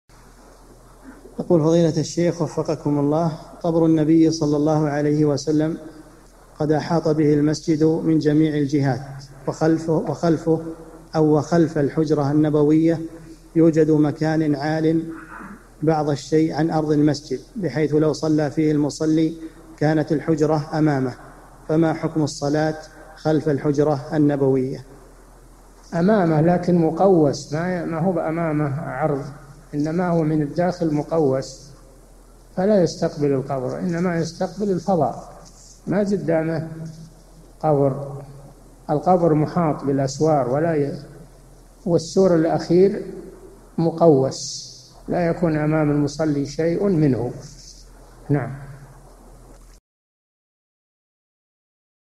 [644 /1350] يوجد خلف الحجرة النبوية مكان عال بعض الشيء عن أرض المسجد فما حكم الصلاة فيه؟صالح الفوزان
-[644 /1350] السؤال: قبر النبي صلى الله عليه وسلم قد أحاط به المسجد من جميع الجهات، وخلف الحجرة النبوية يوجد مكان عال بعض الشيء عن أرض المسجد بحيث لو صلى فيه المصلي كانت الحجرة أمامه، فما حكم الصلاة خلف الحجرة النبوية؟
 الجواب: أمامه لكن مقوس، ما هو بأمامه عرض، إنما هو من الداخل مقوس، فلا يستقبل القبر، إنما يستقبل الفضاء، ما قدامه قبر، القبر مُحاط بالأسوار، والسور الأخير مقوس، لا يكون أمام المصلي شيءٌ منه.
للاشتراك في قناة الشيخ صالح الفوزان
أسئلة وأجوبة من شرح قاعدة جليلة ( 1350 ) مقطع

للاستفسار والإقتراحات والدعم
رئيس الفريق
لمشروع كبار العلماء